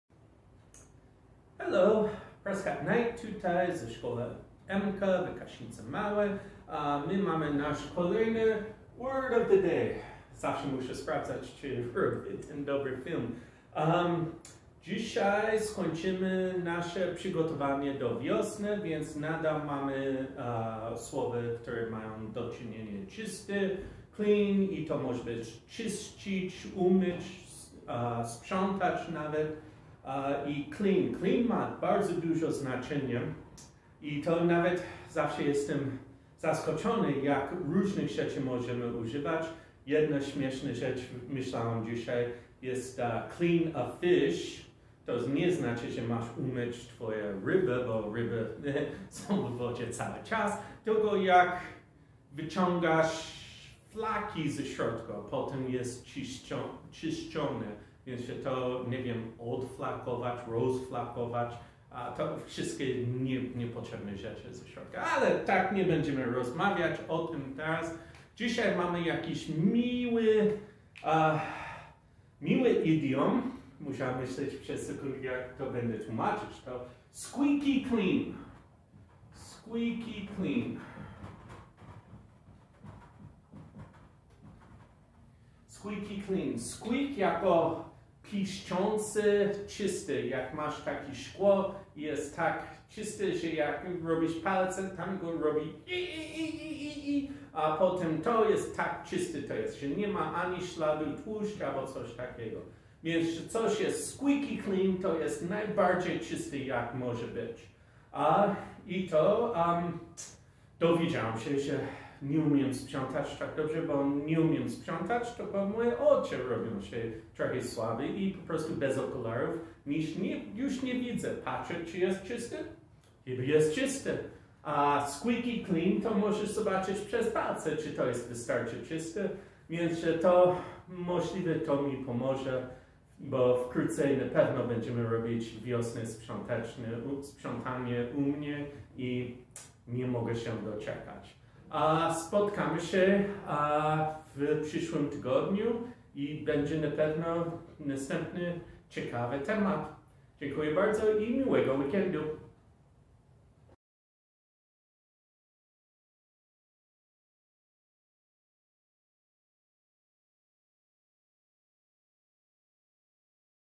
Word of the Day #67 - Squeaky Clean
How do you know when something is really clean? It squeaks!